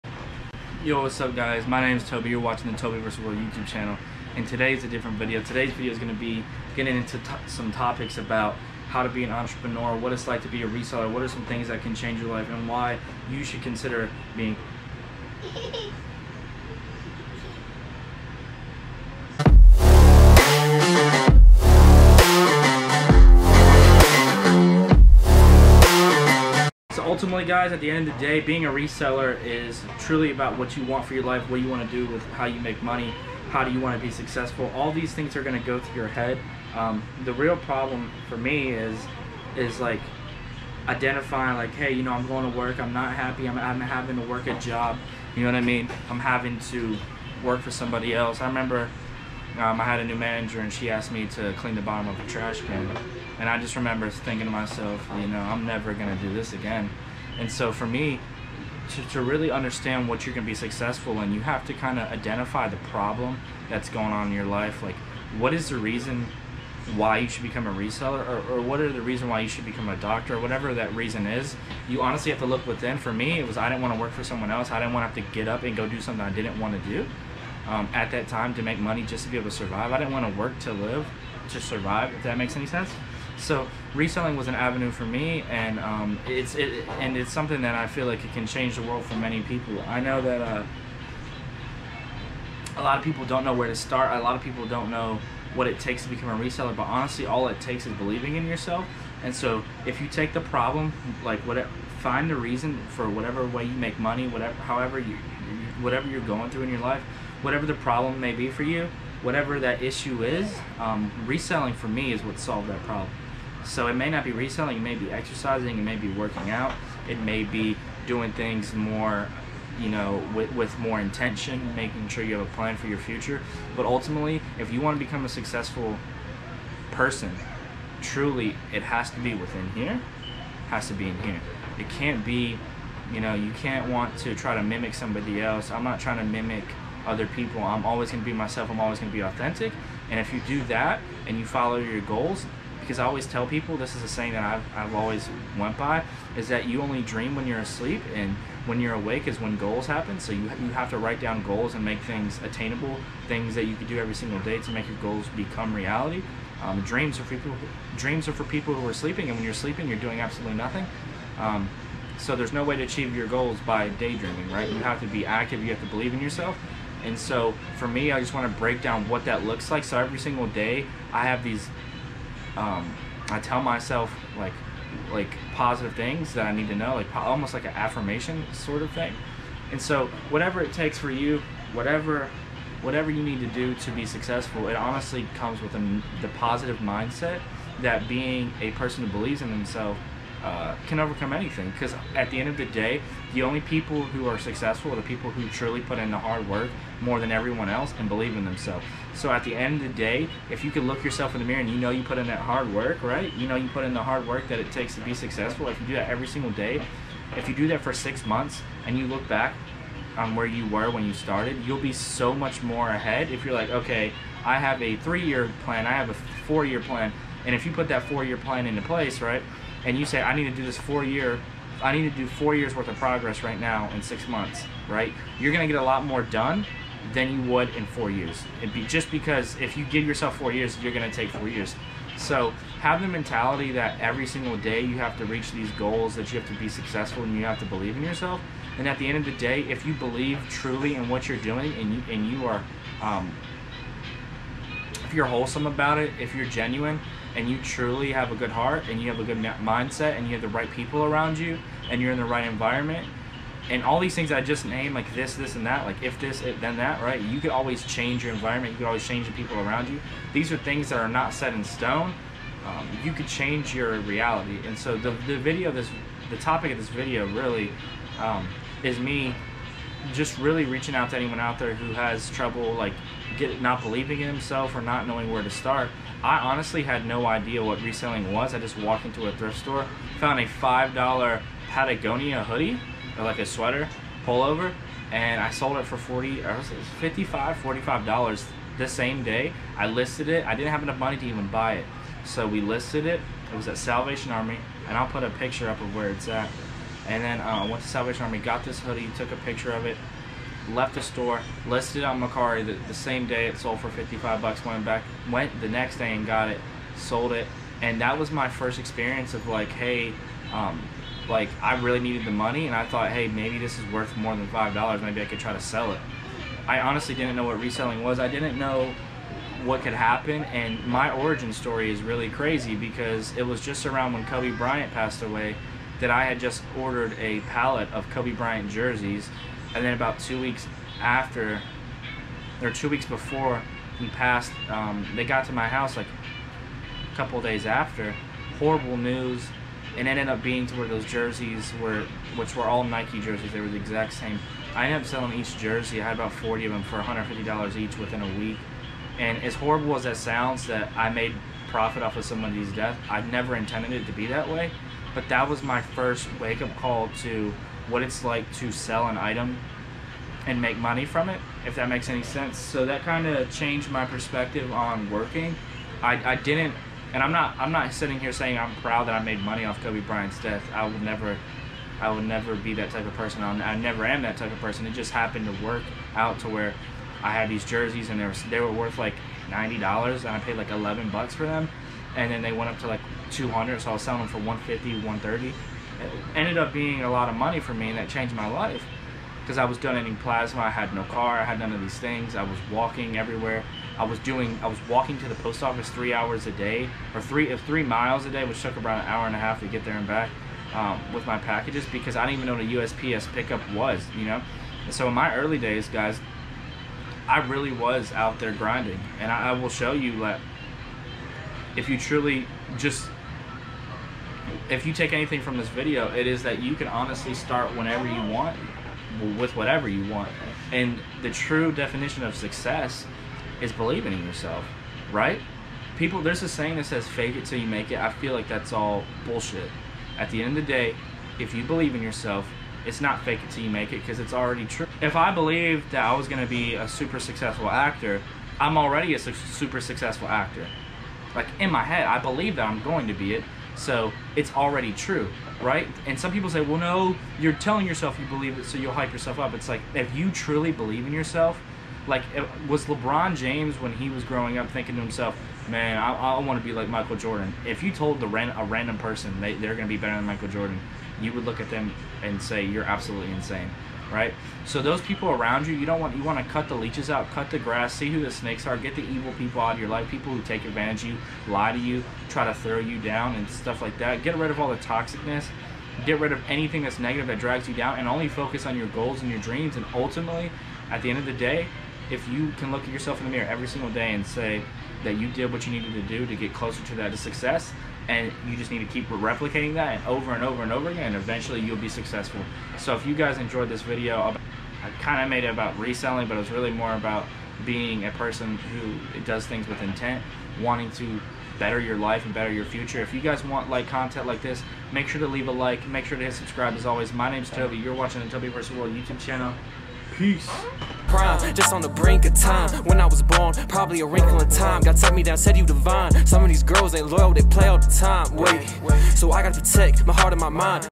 Reselling changed my life
reselling changed my life this is how,  in this video I go through the beginning of my journey, the goal of this video and my channel is to make content about my business my niche but also , teach things that anyone can apply to them